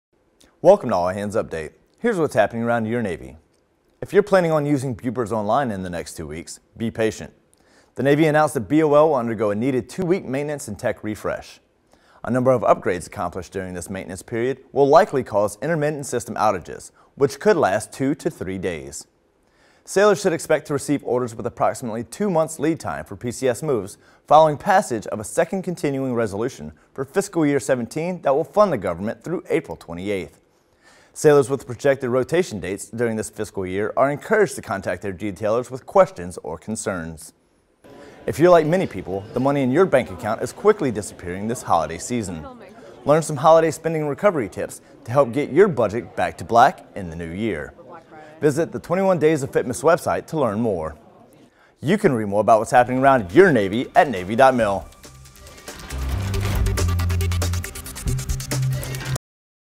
Headlines for Monday, December 19, 2016 (HL-19)
Navy Announces BOL Updates. Continuing Resolution and PCS Order Timelines. FITMAS Finacial.